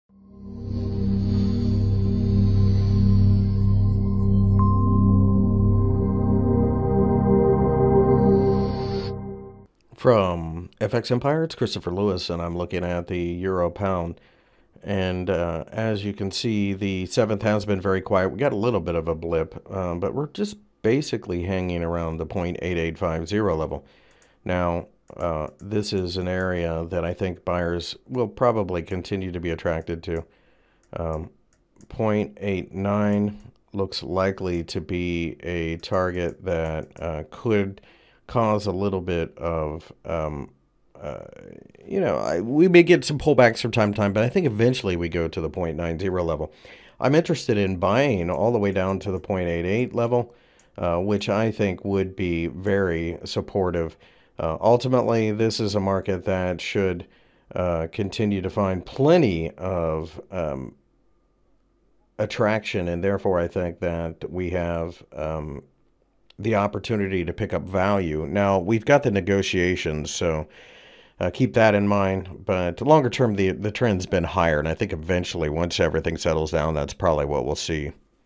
EUR/GBP Technical Analysis for February 08, 2018 by FXEmpire.com
The EUR/GBP pair has bounced around during the trading session on Tuesday, but ultimately looks as if it is finding. For the full article go - EUR/GBP Forecast February 08, 2018, Technical Analysis.Find more information about Forex and Commodities News Technical Analysis Fundamental Analysis and Brokers on FX Empire